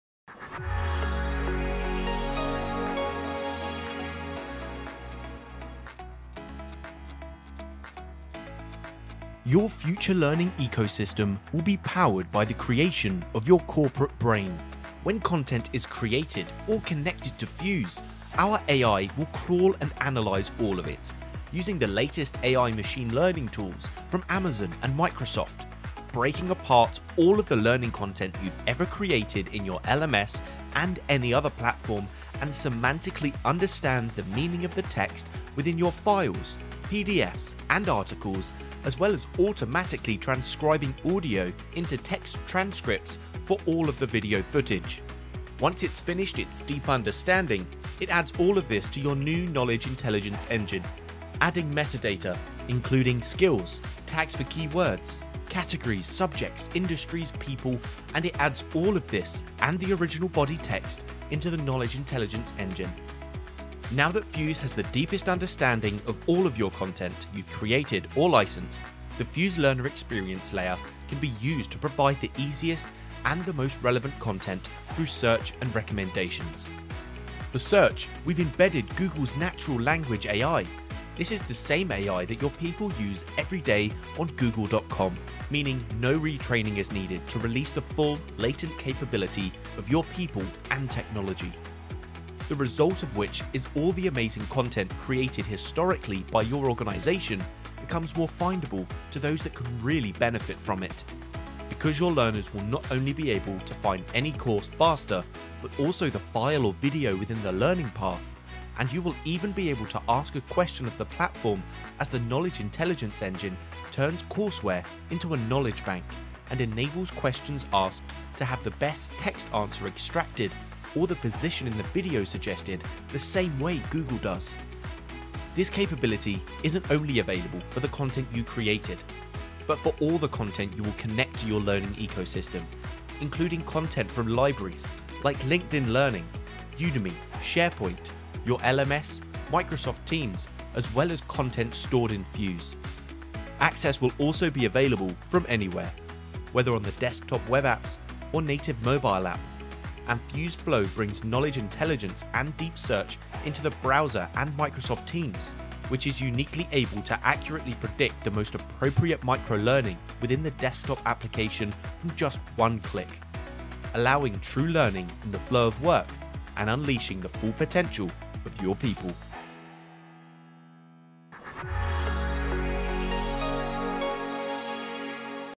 It’s here! Fuse’s new AI-powered Knowledge Intelligence Engine
Fuse is the learning and knowledge platform that gets the knowledge outside of the heads of your experts and into the flow of work. Using the latest machine learning technologies from Google, Amazon and Microsoft, Fuse is helping organisations create a faster route to knowledge.